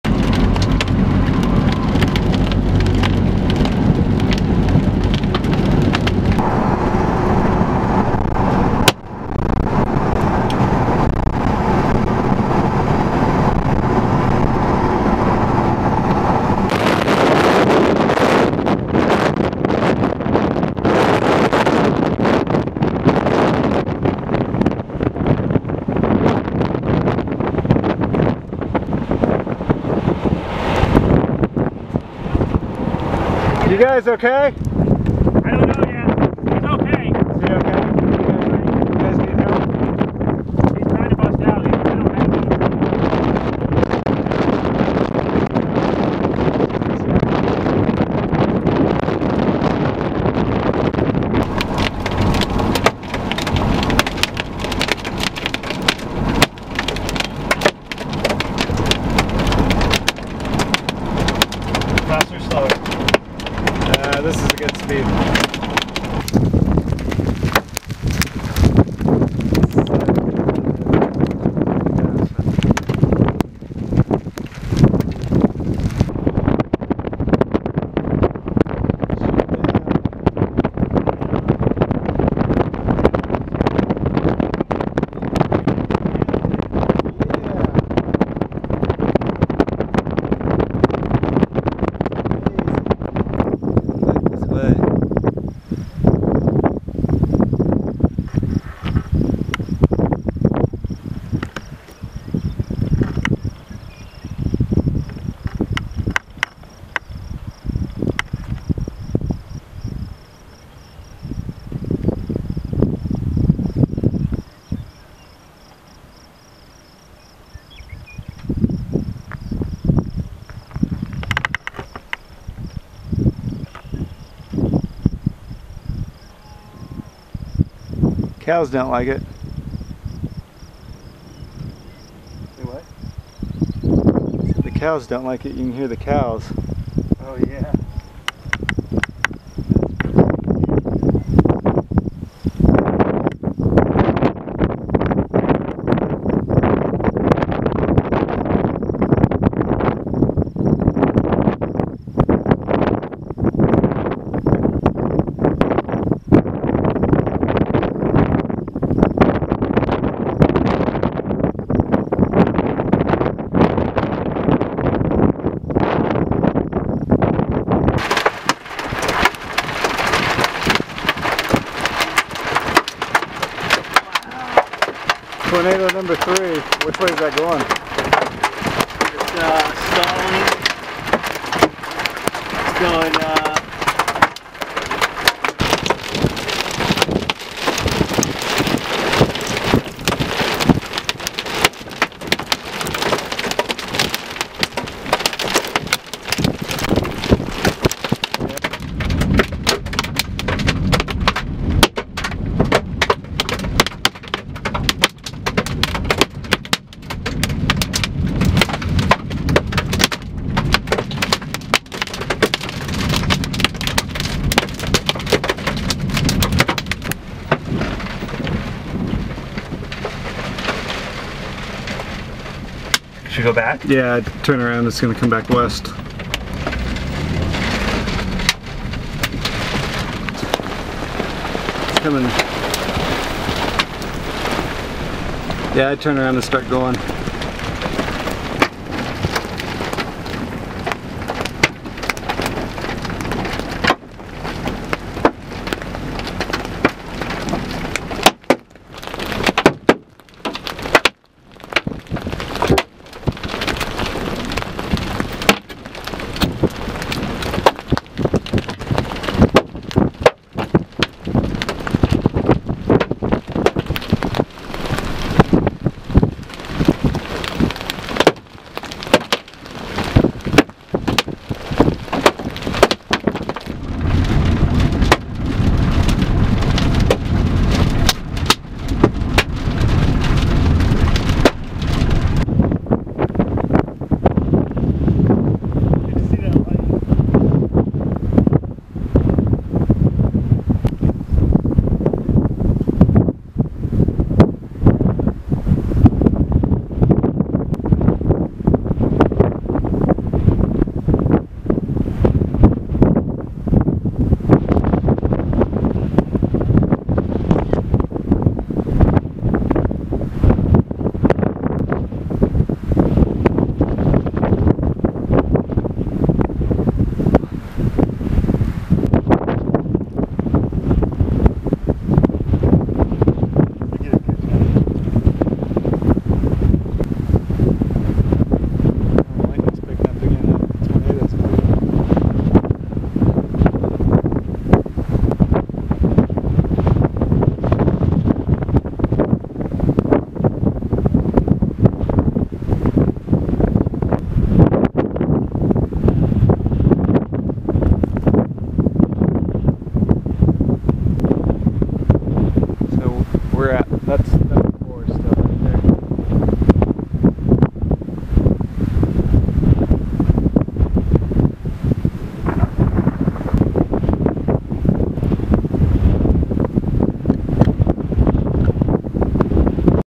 06/17/2014 Nebraska Tornadoes
A supercell moves through Cherry County, Nebraska producing 3 tornadoes. Then another supercell develops to the south and produces a tornado right before sunset.